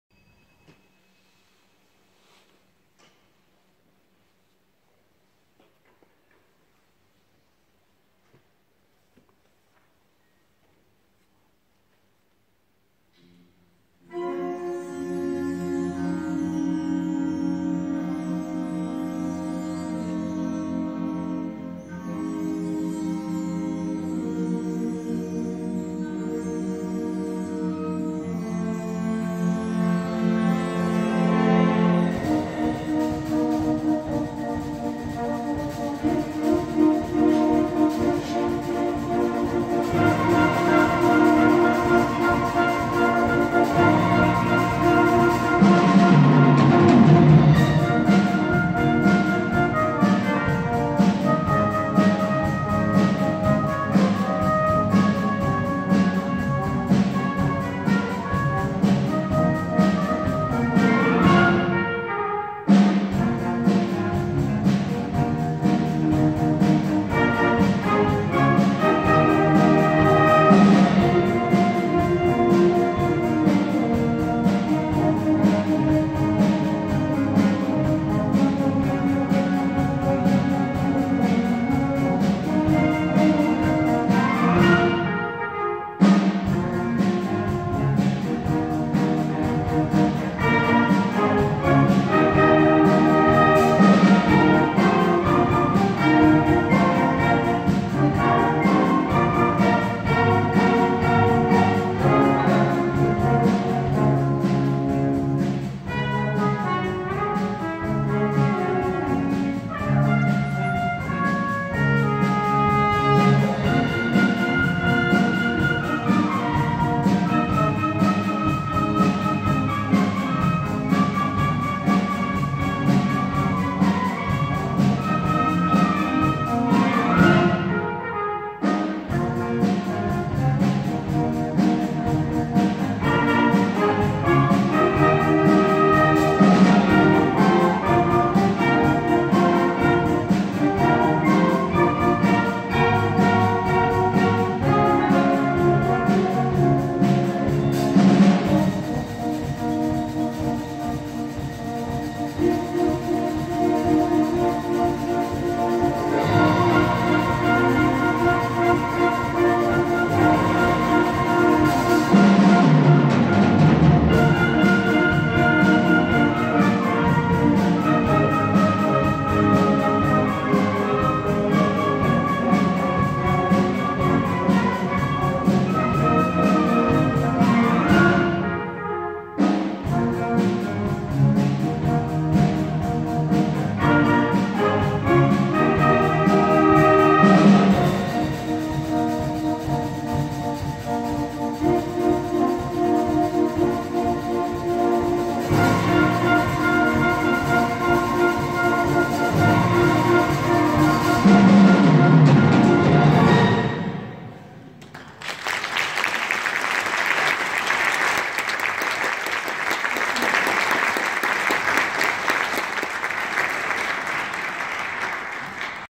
Merici College Junior & Senior Band
Merici College Junior & Senior Band performing at the Australian National Eisteddfod concert